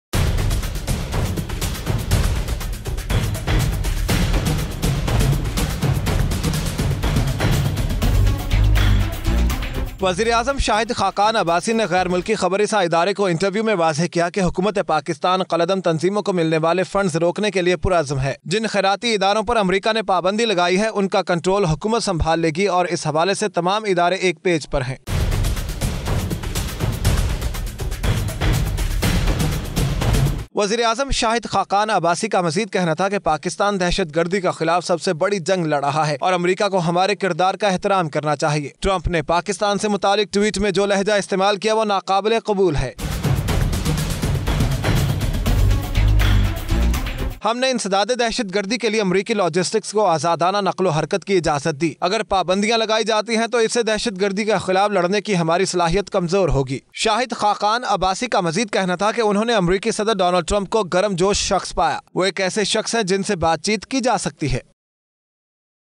Trump's statement regarding Pakistan is unacceptable : Shahid Khaqan Abbasi - 23 January 2018
Trump's statement regarding Pakistan is unacceptable : Shahid Khaqan Abbasi- 23 January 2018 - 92NewsHDPlus